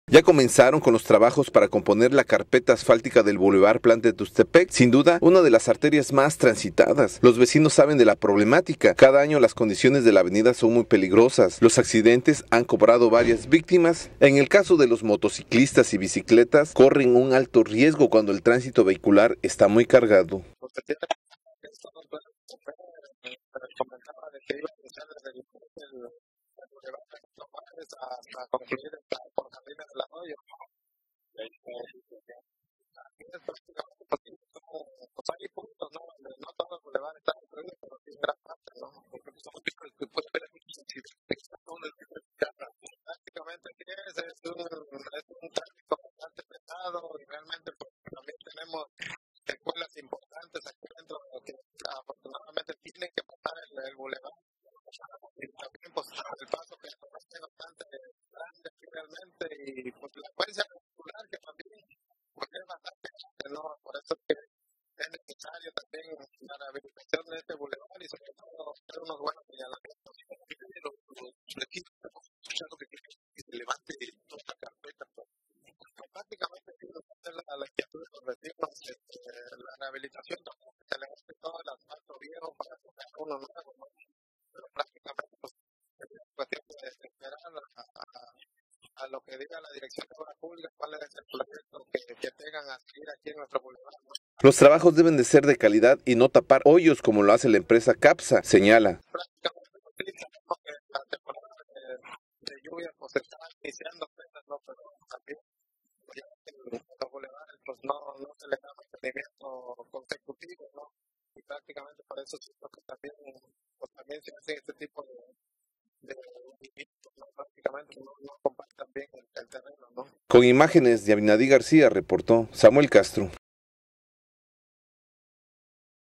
TELE T_RED 38_No solo tapar los baches necesita el Bulevar Plan de Tuxtepec
Ya comenzaron con los trabajos para componer la carpeta asfáltica del bulevar plan de Tuxtepec sin duda una de las arterias más transitadas, los vecinos saben de la problemática, cada año las condiciones de la avenida son muy peligrosas, los motociclistas y bicicletas corren un alto riesgo cuando el tránsito vehicular está muy cargado.